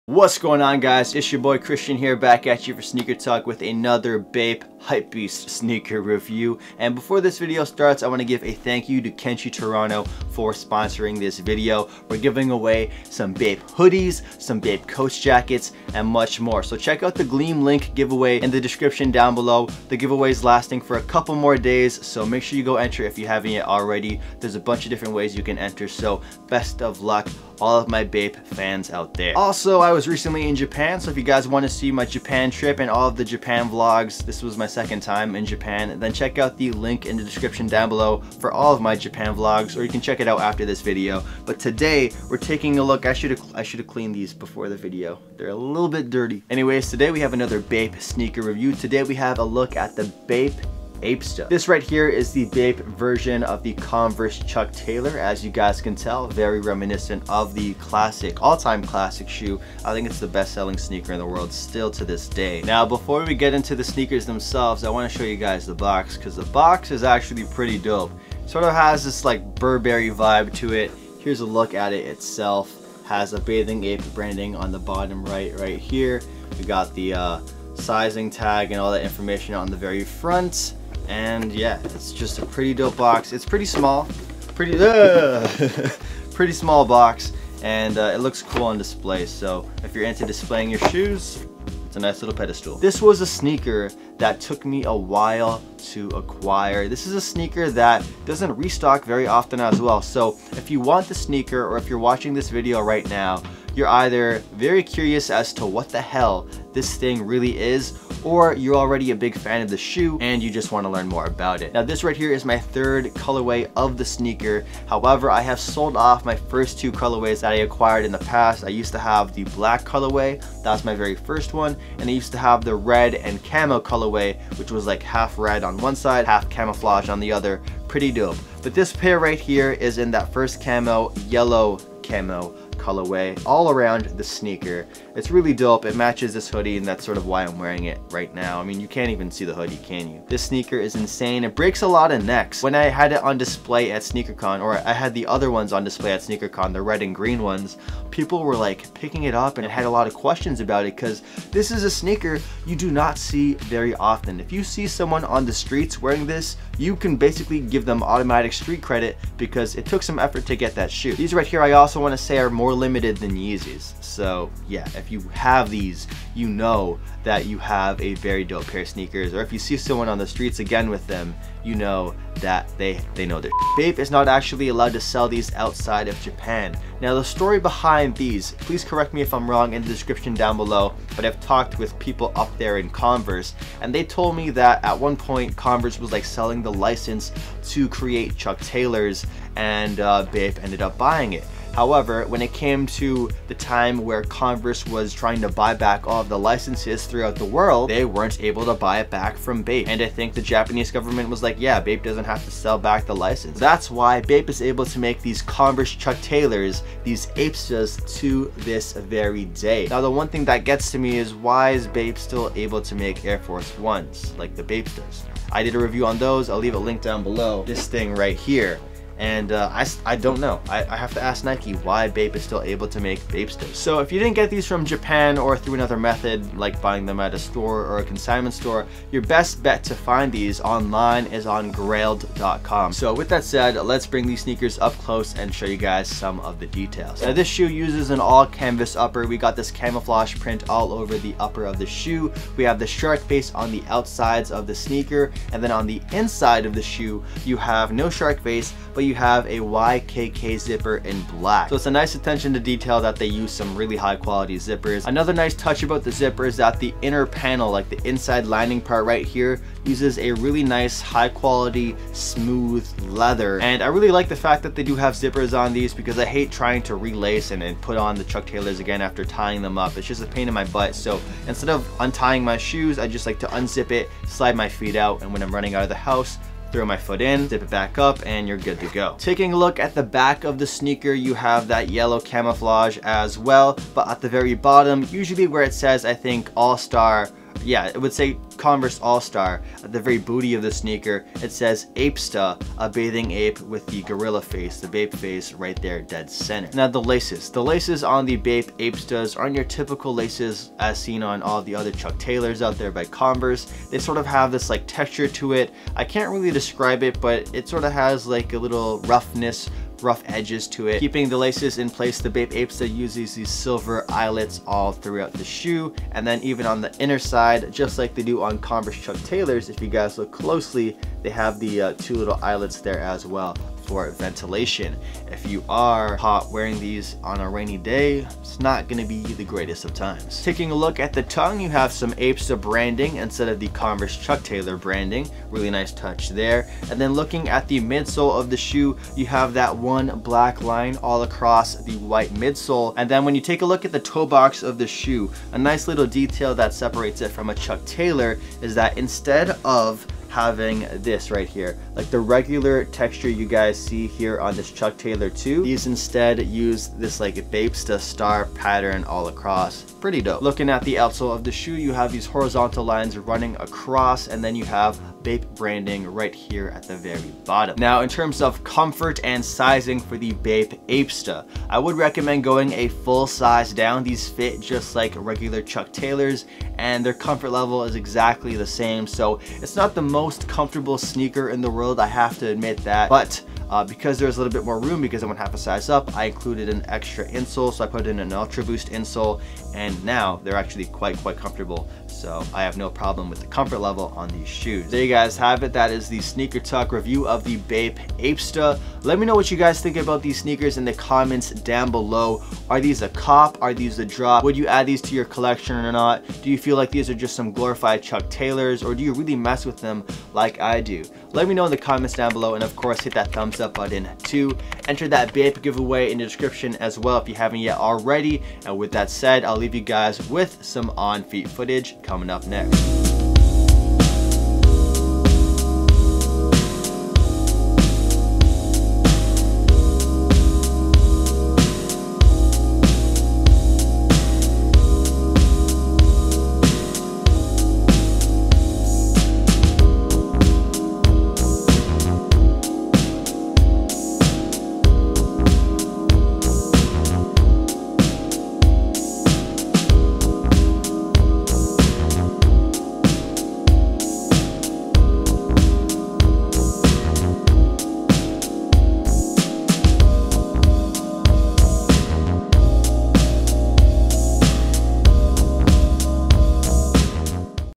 $300 CONVERSE? BAPE APESTA SHARK CAMO REVIEW | On-Feet
Today on SneakerTalk we review the BAPE APESTA SHARK CAMO!